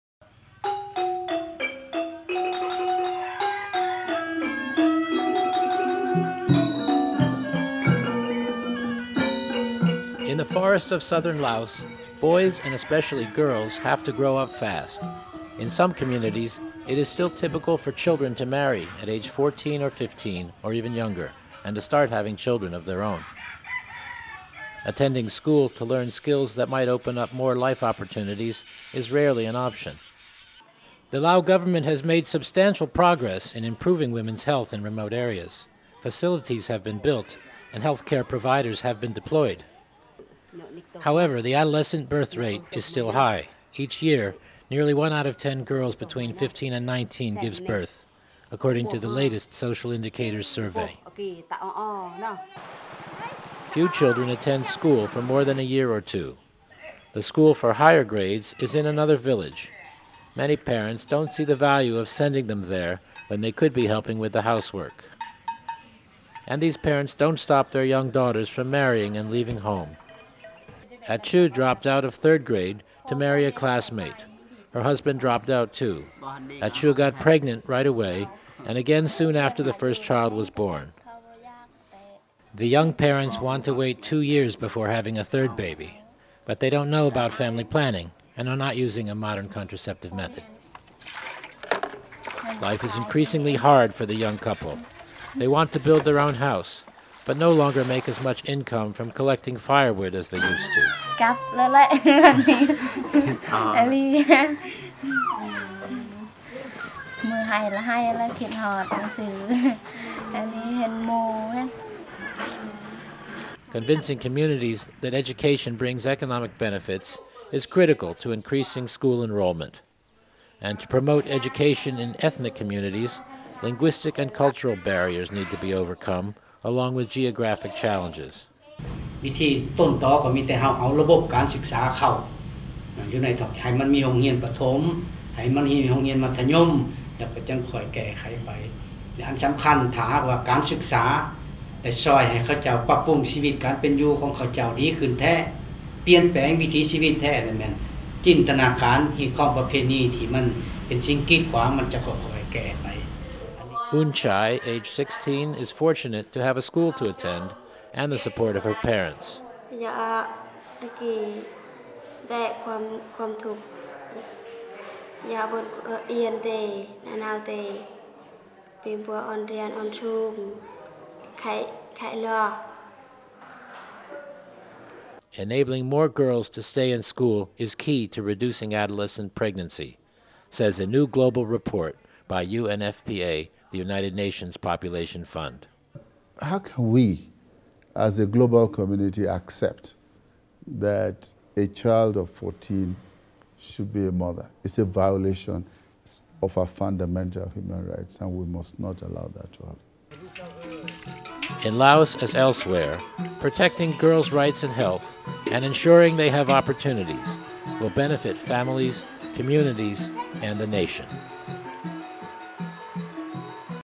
UNFPA apoia empoderamento de meninas e adolescentes no Laos
No Laos, país localizado no sudeste asiático, muitas meninas se casam e engravidam precocemente. O Fundo de População das Nações Unidas (UNFPA) aponta a educação como um mecanismo essencial para reduzir a taxa de gravidez na adolescência.